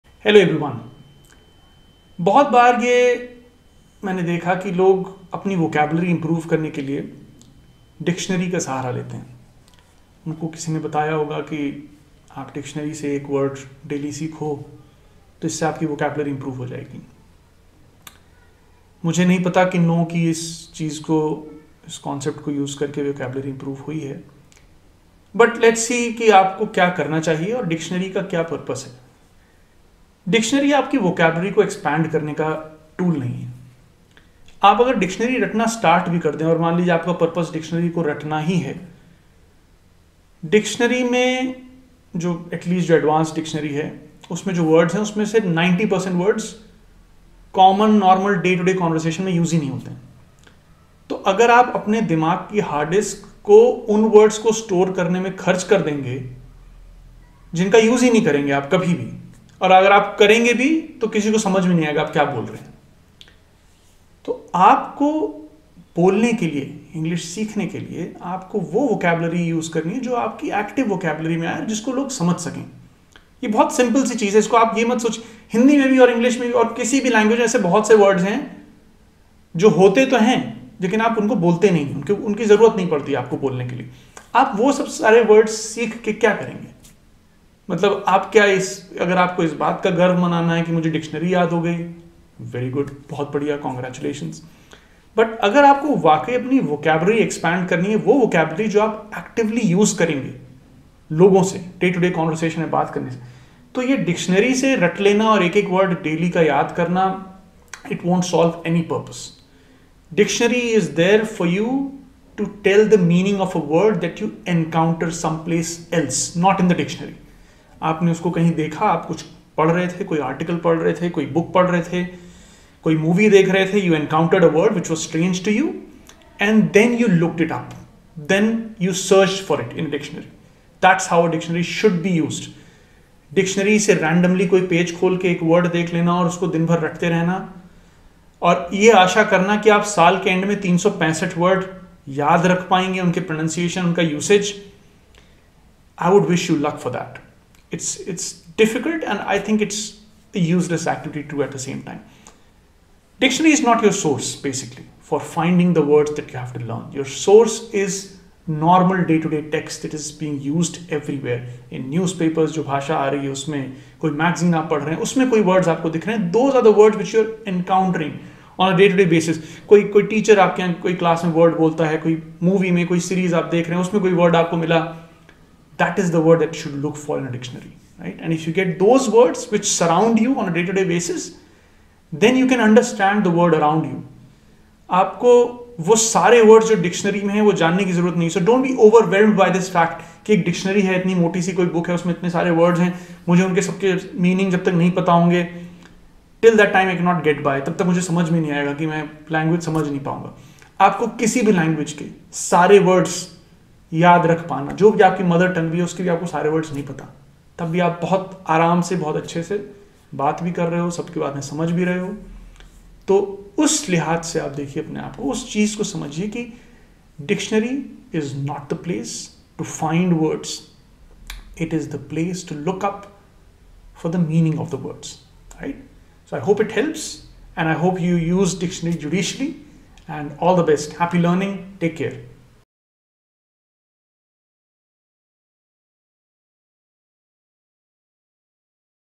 How (not) to use a dictionary | Kaise use karein Dictionary | English Speaking tips
Can a dictionary be used to expand your vocabulary?
What is the purpose of a Dictionary?
How to use a dictionary to help you improve your vocabulary?
This video talks about it in brief.